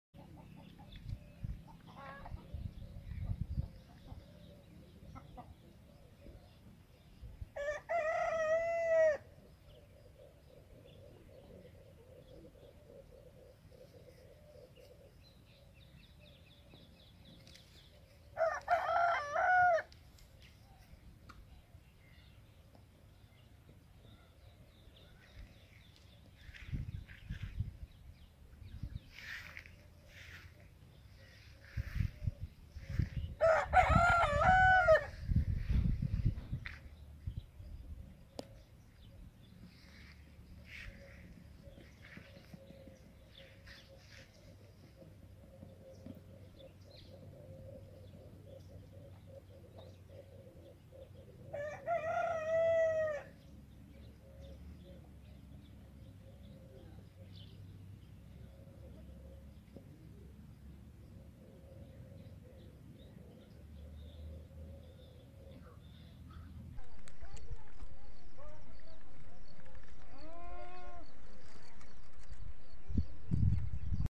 Baby Goat Milking Goats At Goat Farming | Bakri Ka Baccha
cute baby goat milking in goat farming of the goat. goat milking is very important for baby goats early days. goat milking video of the baby goat in my farm.